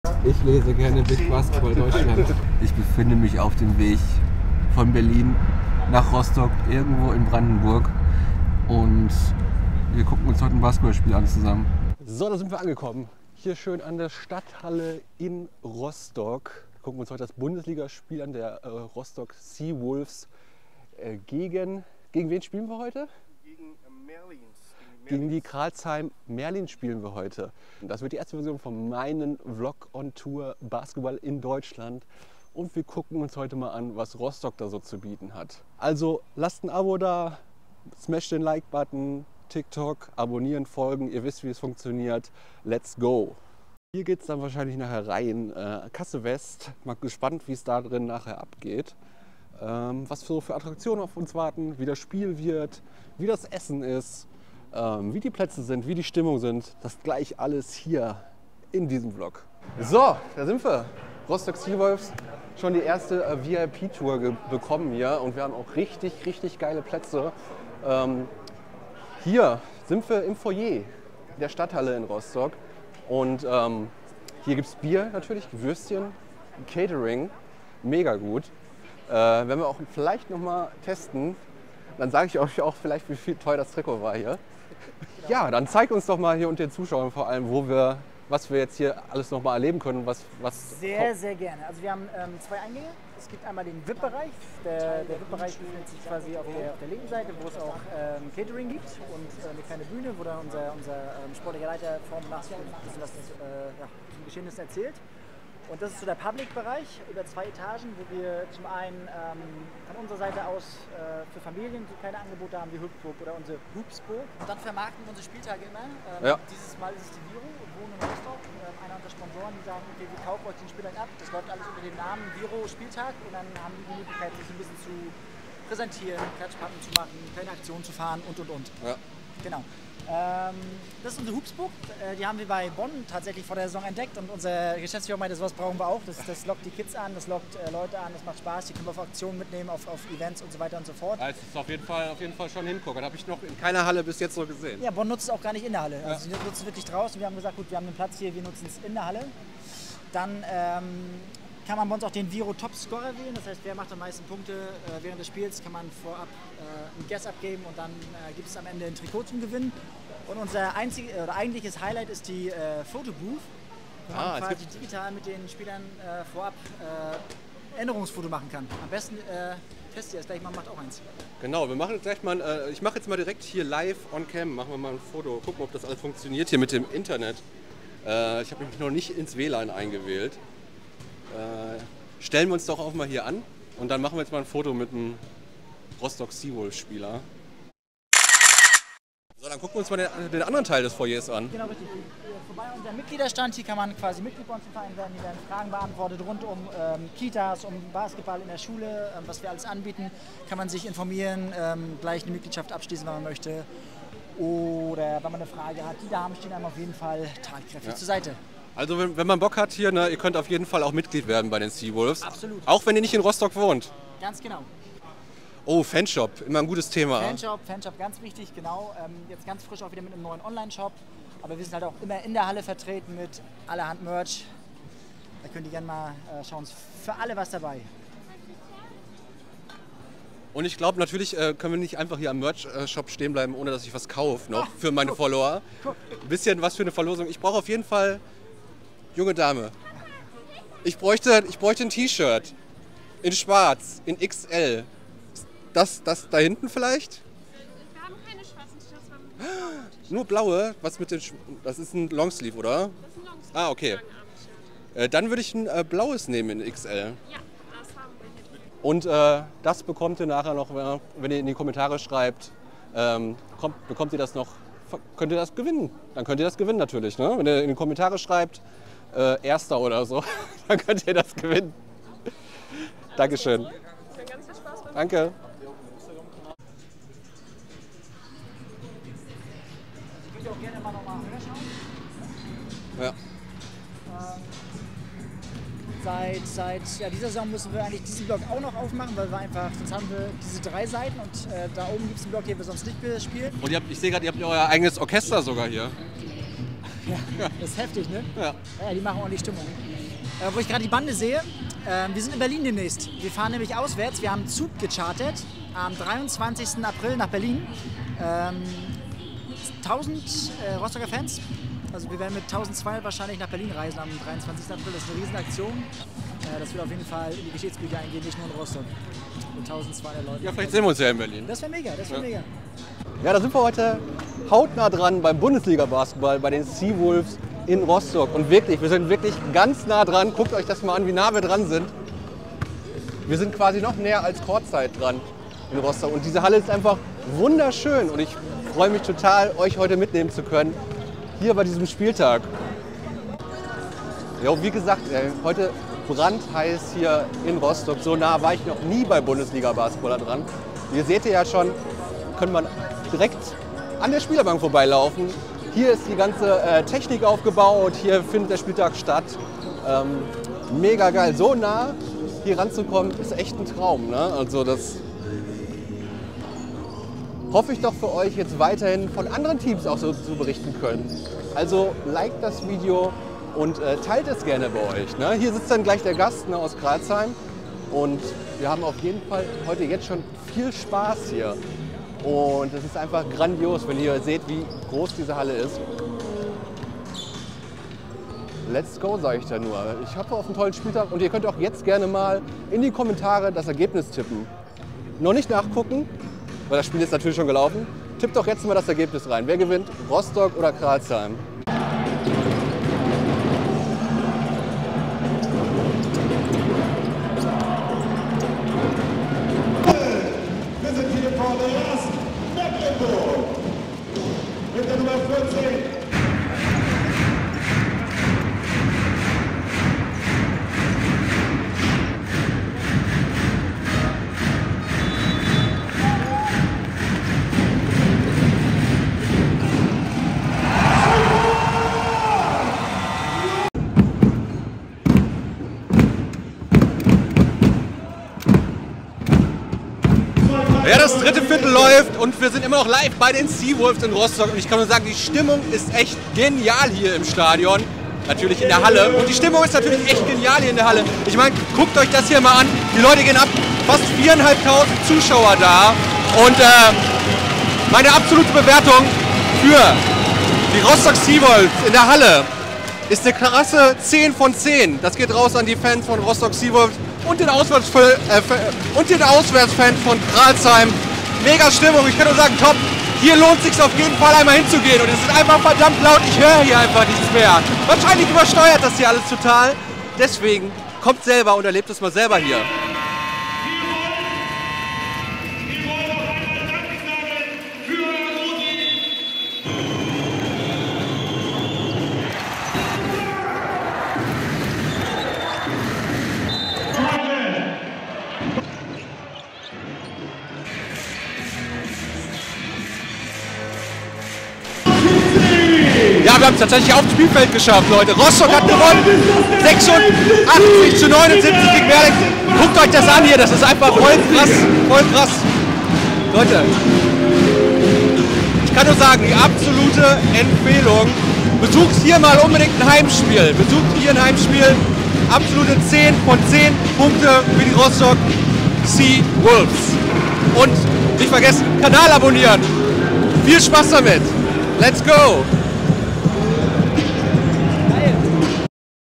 Basketball Bundesliga Hautnah: Ein Tag bei den Rostock Seawolves
In diesem Vlog nehme ich euch mit auf einen aufregenden Basketball Bundesliga Spieltag bei den Rostock Seawolves. Wir erleben hautnah, wie die Mannschaft sich auf das Spiel gegen die Crailsheim Merlins vorbereitet und welche Emotionen auf dem Spielfeld und den Rängen herrschen.

Wir werfen einen Blick hinter die Kulissen der Seawolves und bekommen exklusive Einblicke in das Training, die Kabine und die Vorbereitungen auf den Spieltag. Ihr erfahrt, wie die Spieler sich motivieren und welche Strategien sie verfolgen, um ihre Gegner zu besiegen.

Die Basketball Bundesliga steht für hochklassigen Sport und spannende Duelle, bei denen es um jede Sekunde und jeden Punkt geht. Ob als Fan oder Neuling, ein Besuch lohnt sich immer und bietet ein unvergessliches Erlebnis.

Ich danke den Rostock Seawolves für die Möglichkeit, diesen Vlog zu drehen und hoffe, euch damit einen Einblick in die faszinierende Welt des Bundesliga Basketballs gegeben zu haben.